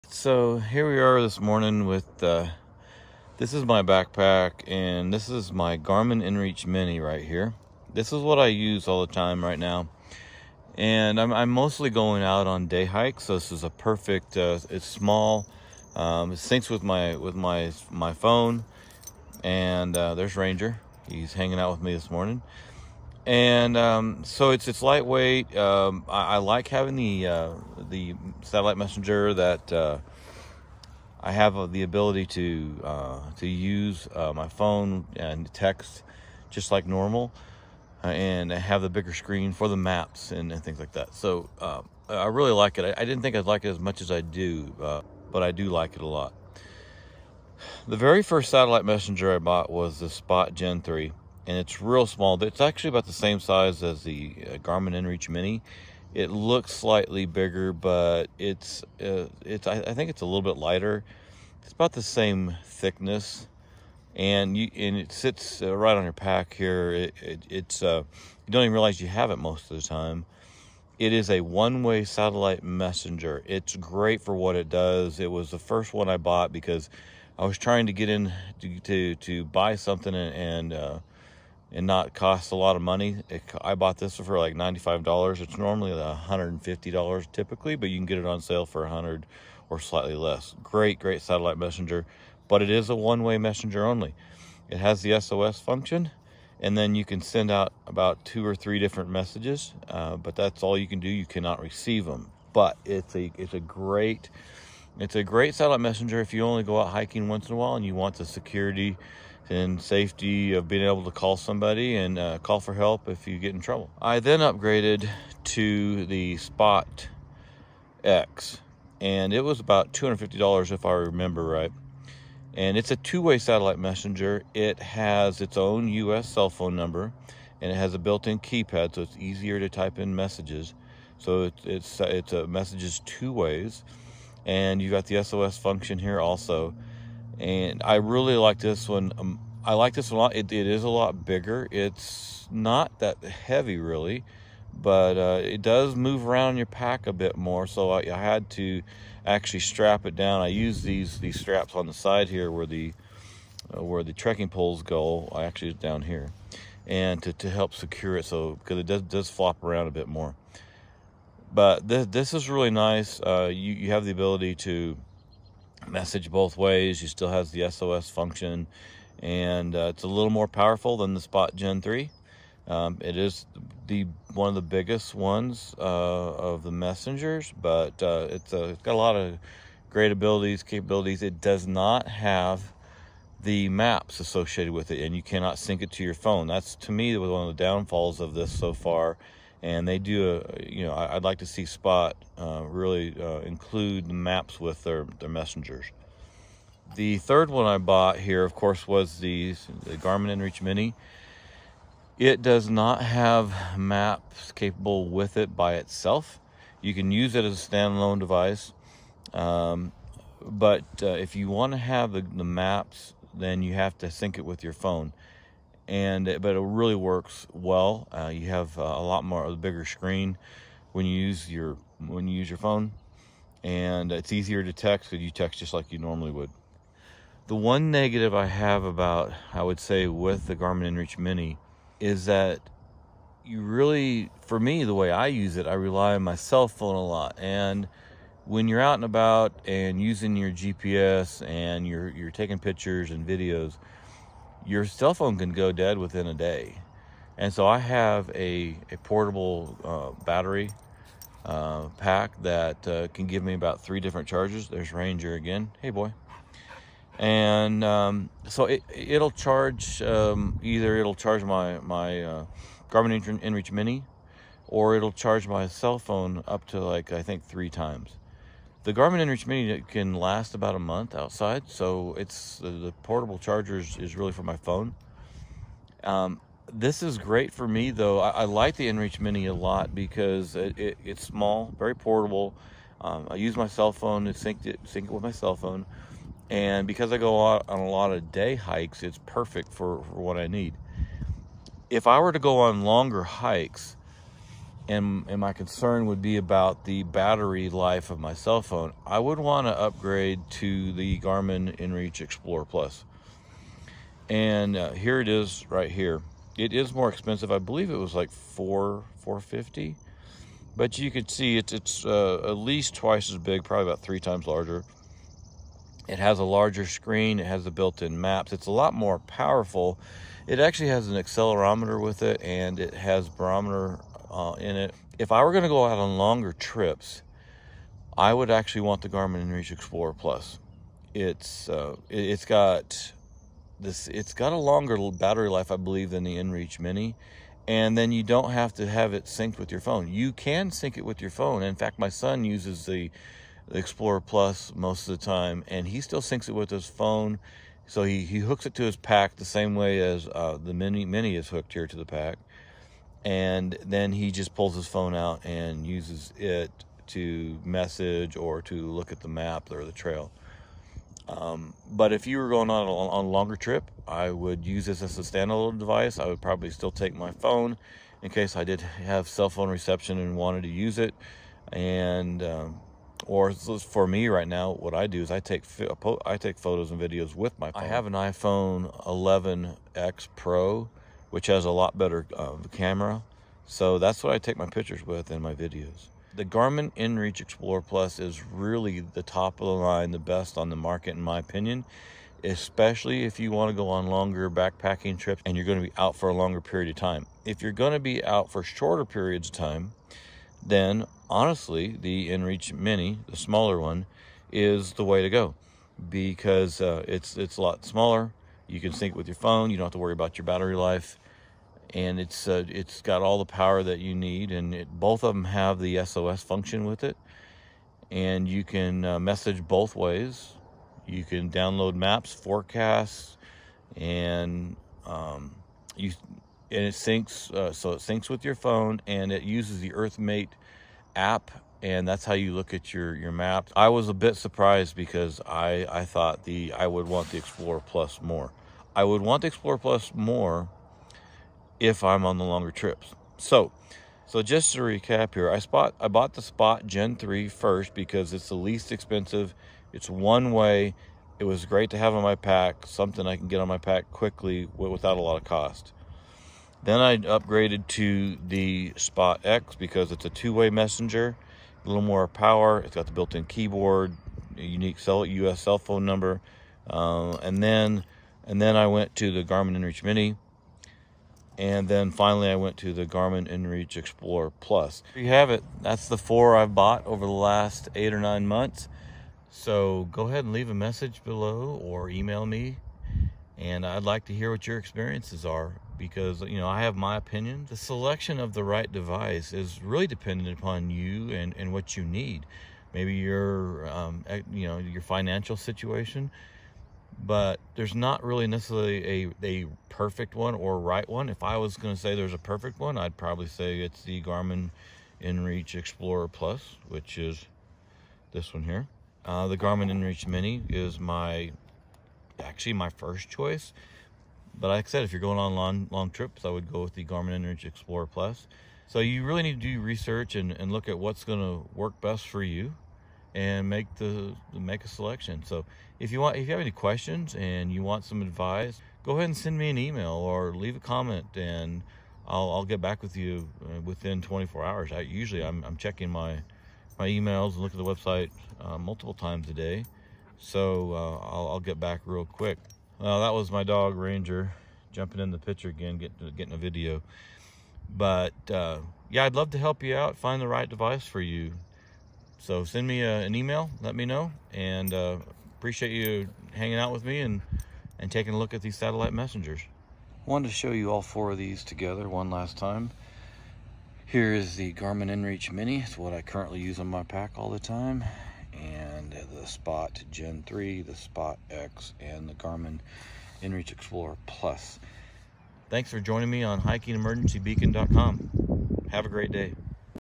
Satellite Messenger Devices
Check Out These 4 Satellite Messengers I Have Bought and Tested Over the Last Year. One of These May be the Right One for You. Let Me Show You What I Have Found Out.

🏬 Amazon PRODUCT LINKS:
Garmin inReach Mini
Garmin inReach Mini 2
Garmin inReach Explorer
SPOT X
SPOT Gen 3: (only available used)
SPOT Gen 4
Garmin GPSMAP 66i
Garmin GPSMAP 67i
Garmin inReach Messenger
Garmin Montana 700i